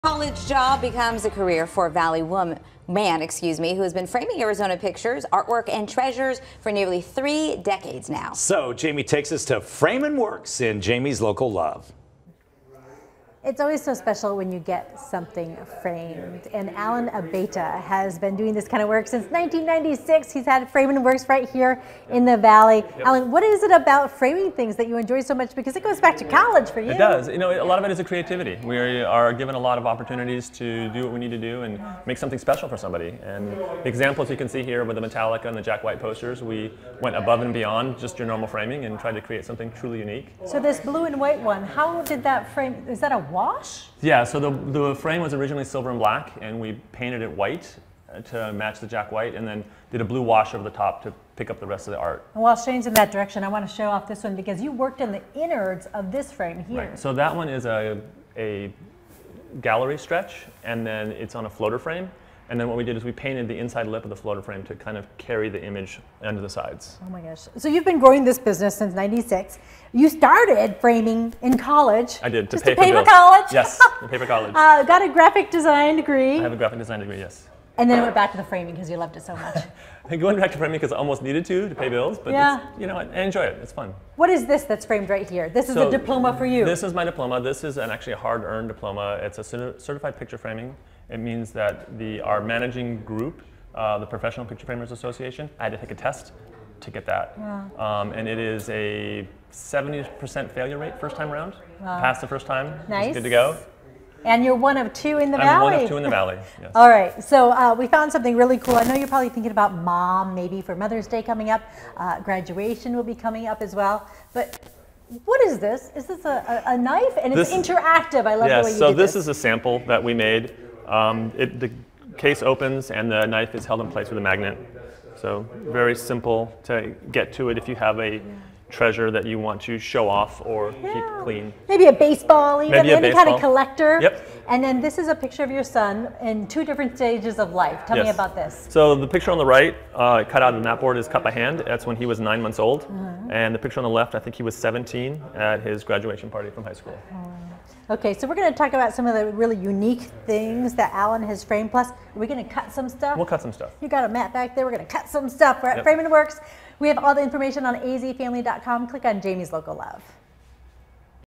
Local business owner framing Arizona for 3 decades
Starting with Framin' Works in Phoenix in 1996, Alan Abeyta has also opened Best Lil Frame Shop in Scottsdale.